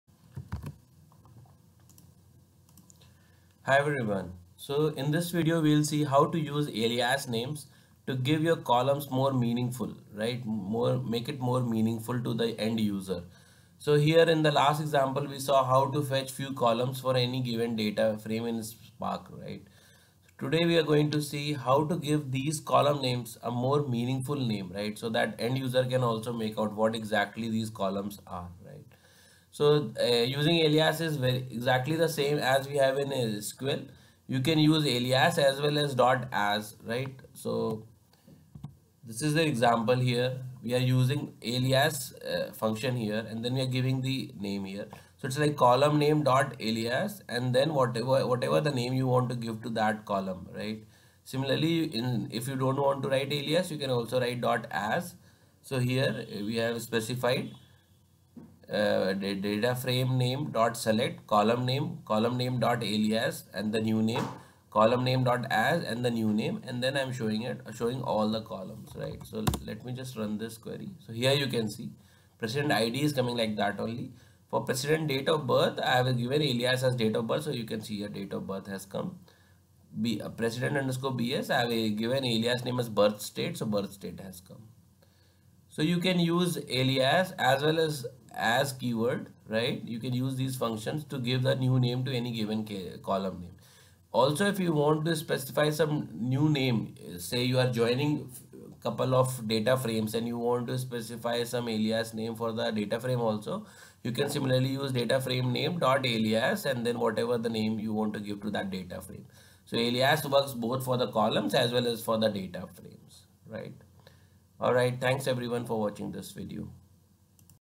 Spark Dataframe Alias As
You can specify ALIAS name for any column in Dataframe. Also you can specify Alias names for any dataframe too in Spark. Alias serves two purpose primarily:
1) They give more meaningful name to the column or dataframe which is easily understood by end user.
2) You can specify different alias names to same object if required eg in self joins.